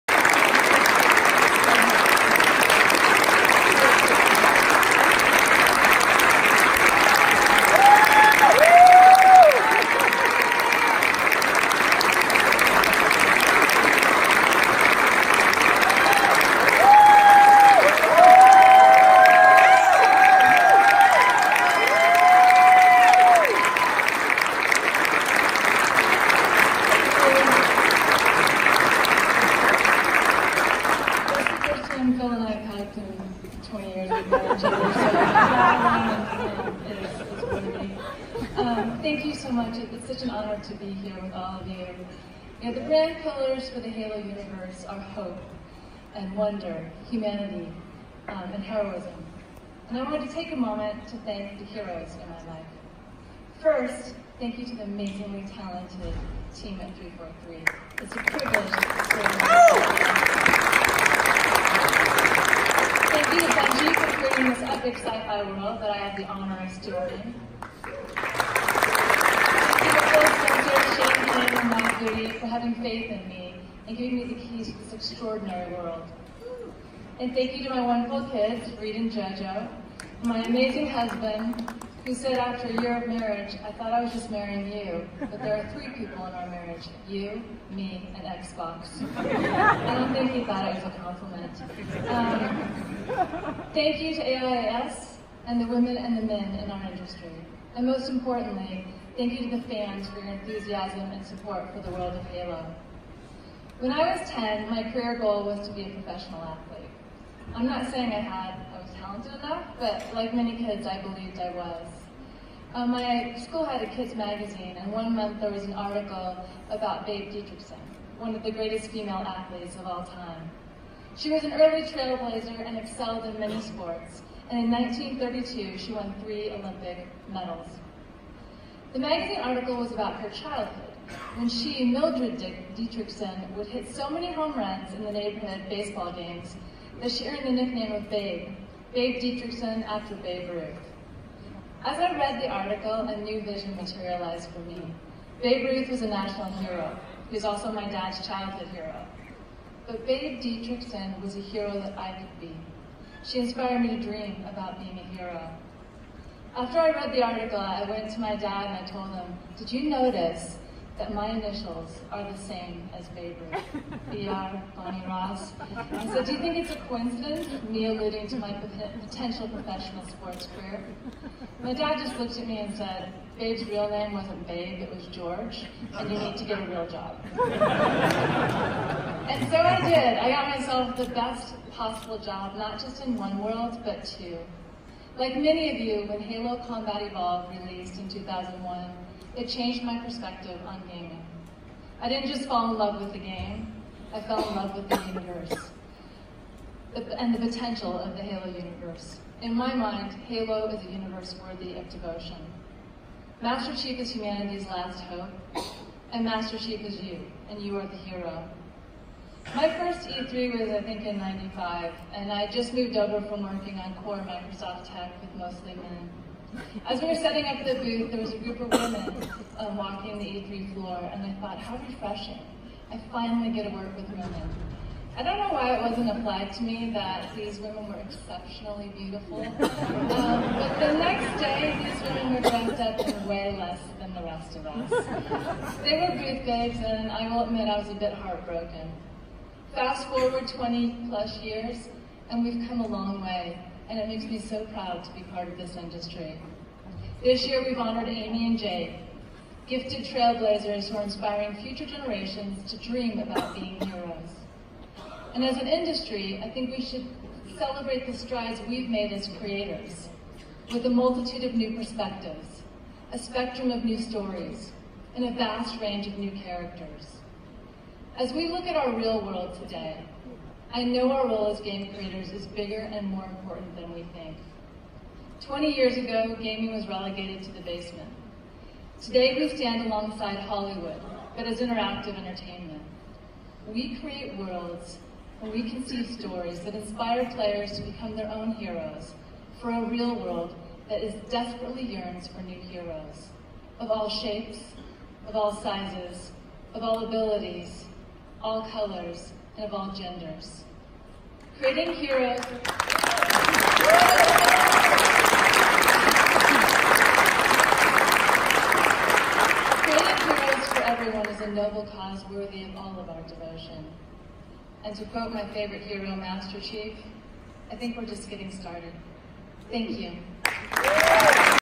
Bonnie Ross Hall of Fame speech at DICE Awards
Bonnie Ross gives her acceptance speech for the AIAS Hall of Fame Award.